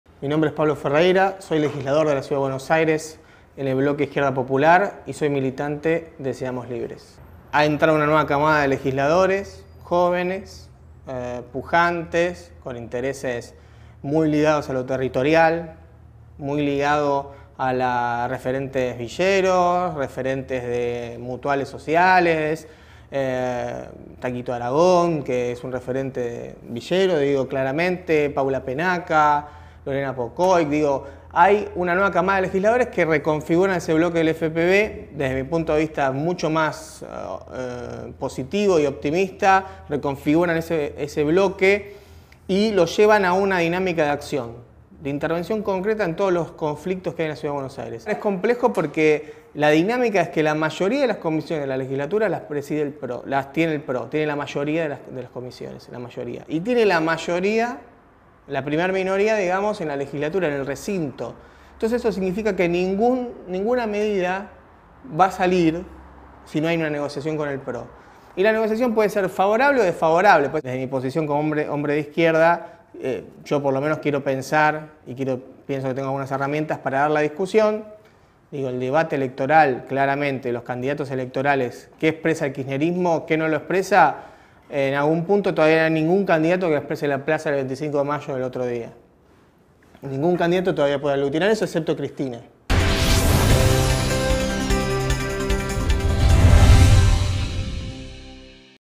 Pablo Ferreyra:"Buenos Aires es una ciudad exclusiva que se mira todo el tiempo el ombligo"
El diputado por el Bloque Izquierda Popular dijo también que la legislatura porteña "es adversa a los sectores populares y que aún no hay un candidato presidencial que pueda expresar la plaza del 25 de mayo último "excepto Cristina".